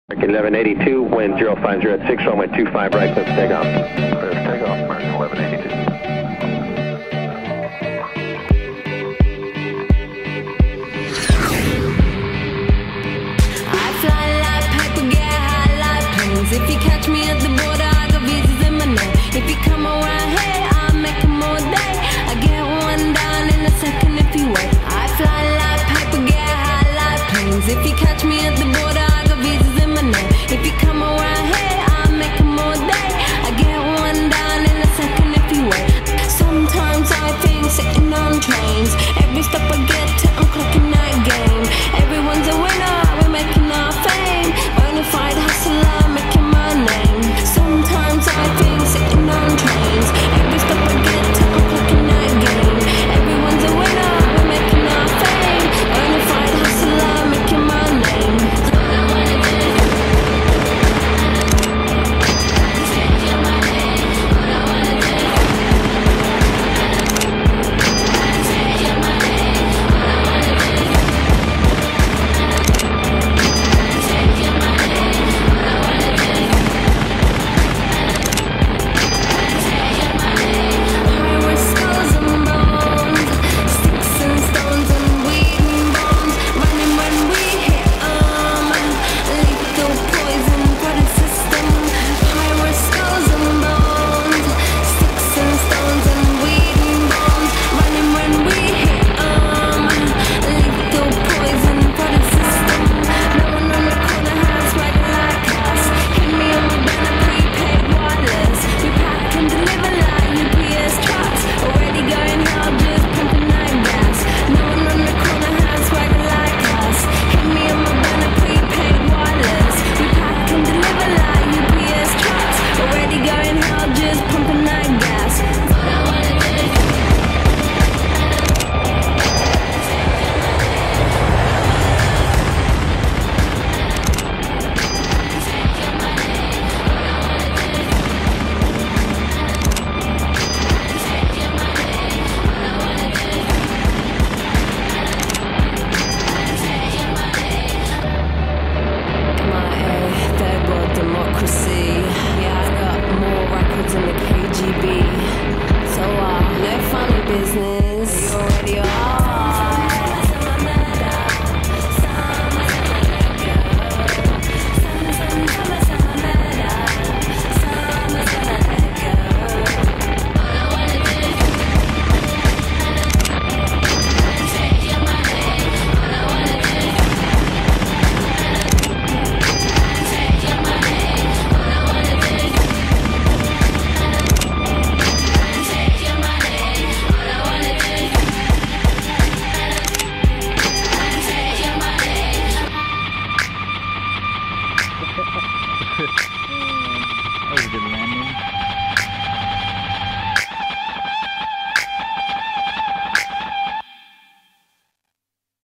Power Parachuting
The craziest airplane ever..imagine sitting in a go-kart a couple of hundred feet high & racing...the best adrenaline rush possible...awesome time.